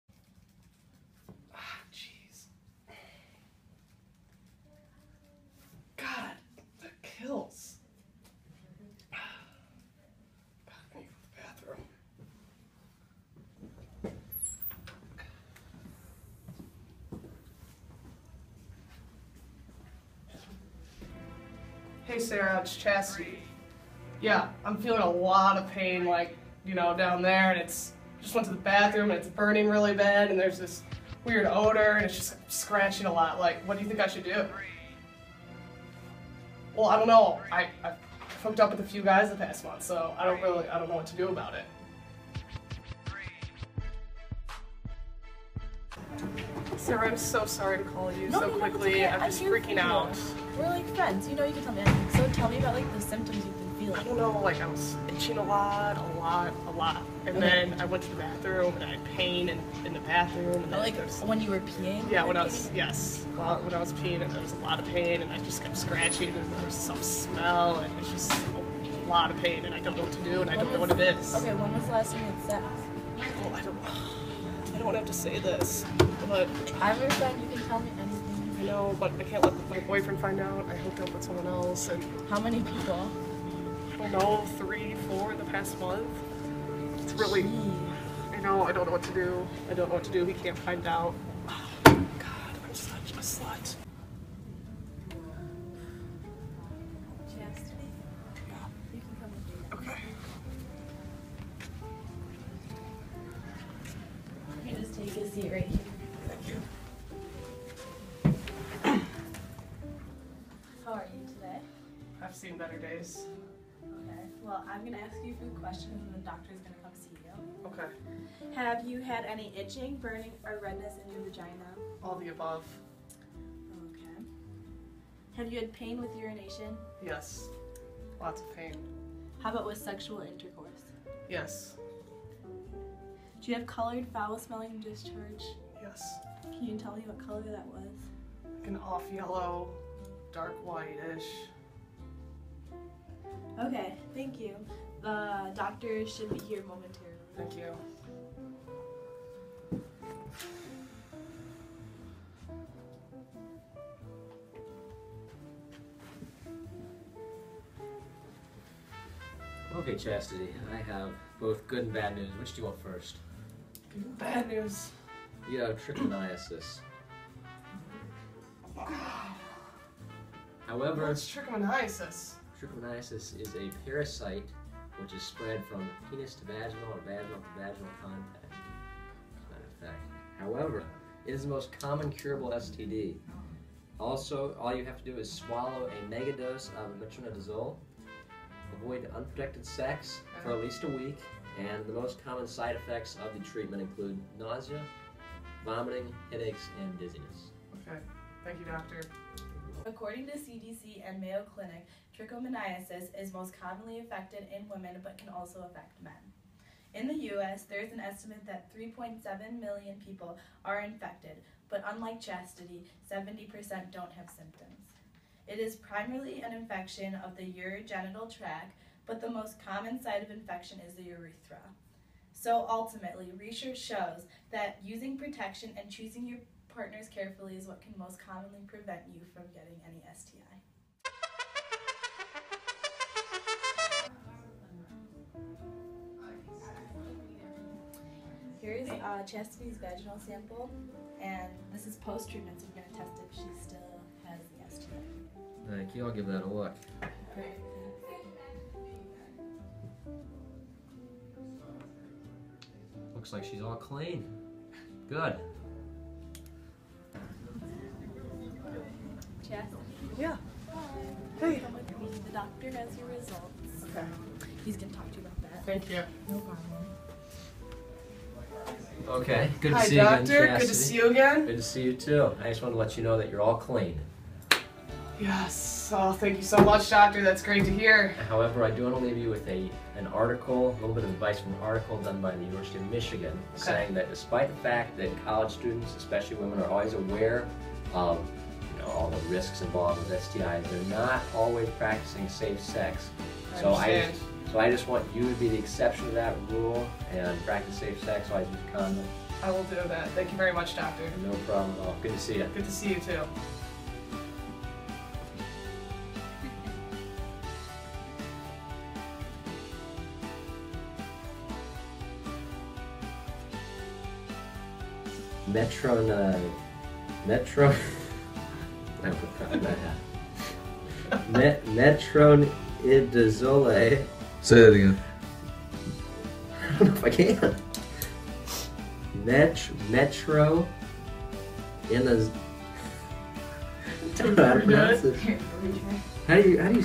Trichomoniasis
Scholarly sources are quoted along with scientific evidence of trichomoniasis as you are brought along on Chastity's journey to getting cured.